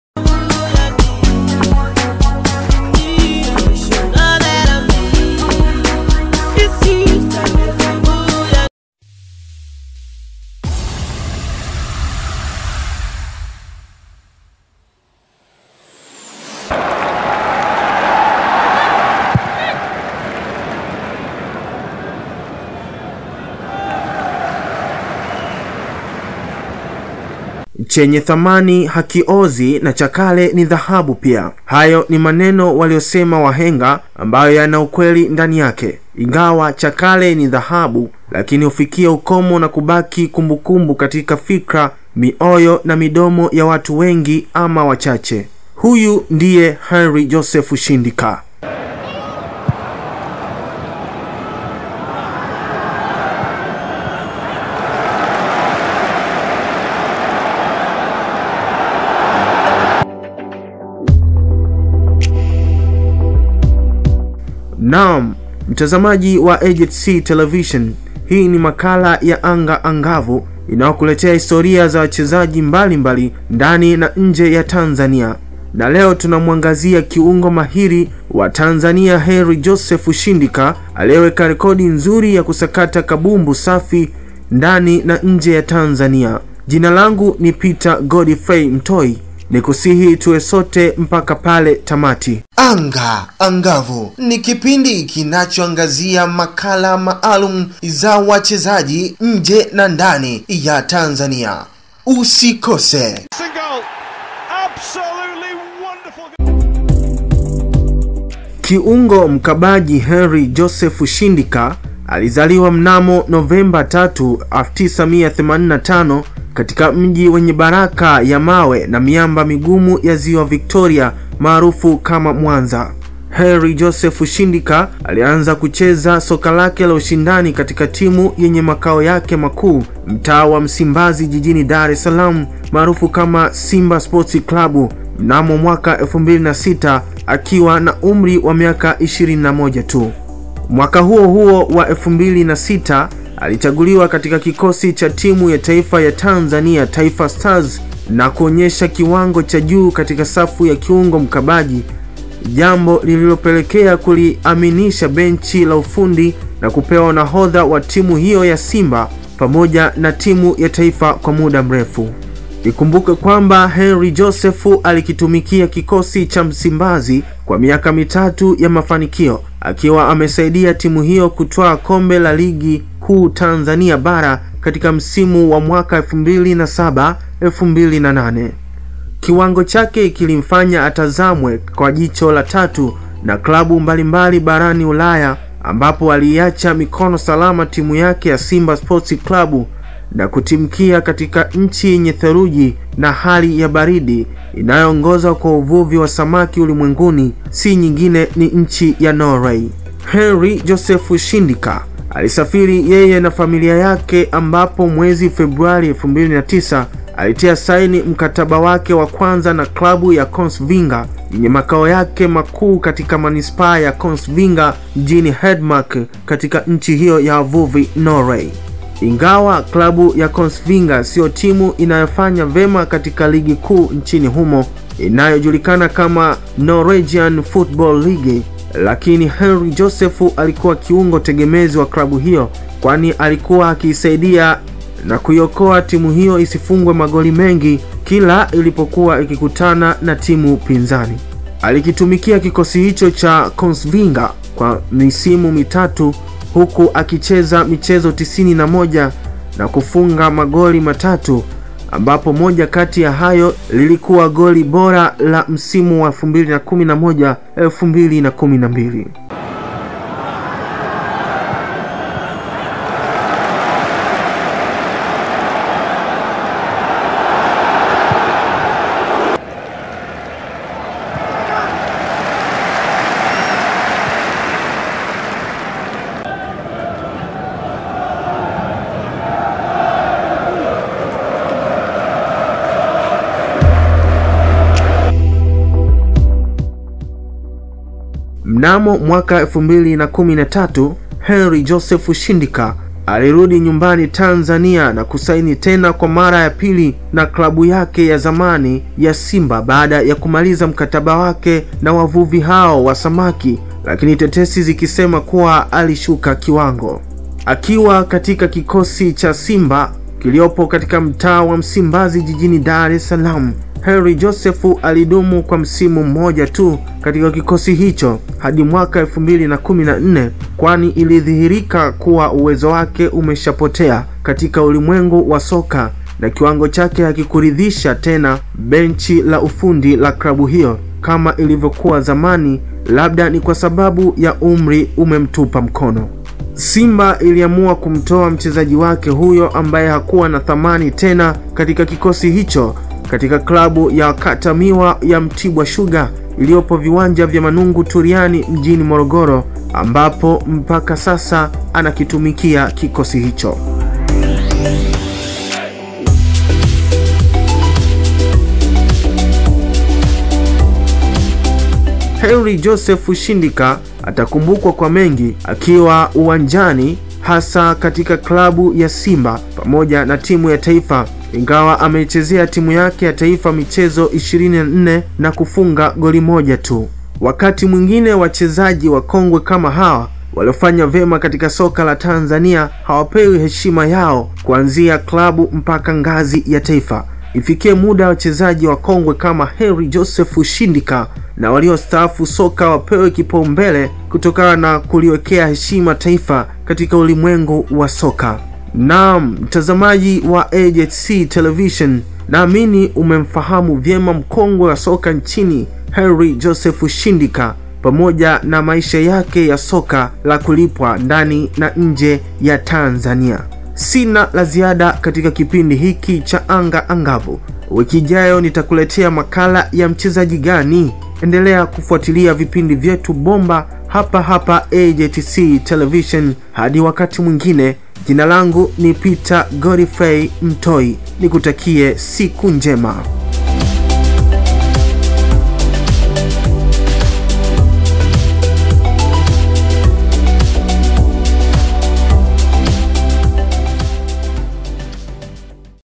MAKALA YA ANGA ANGAVU YA MCHEZAJI MKONGWE WA TANZANIA HENRY JOSEPH SHINDIKA
Hii ni Makala ya Anga Angavu na leo tunamuangazia mchezaji nguli na mkongwe si mwingine muite Henry Joseph Shindika aliyezichezea klabu mbalimbali ndani na nje ya Tanzania ikiwamo Simba SC,Mtibwa Sugar na Kongsvinger ya nchini Norway pia alisakata kabumbu katika timu yake ya taifa Taifa Stars na kujizolea umaarufu mkubwa katika safu ya Kiungo Mkabaji.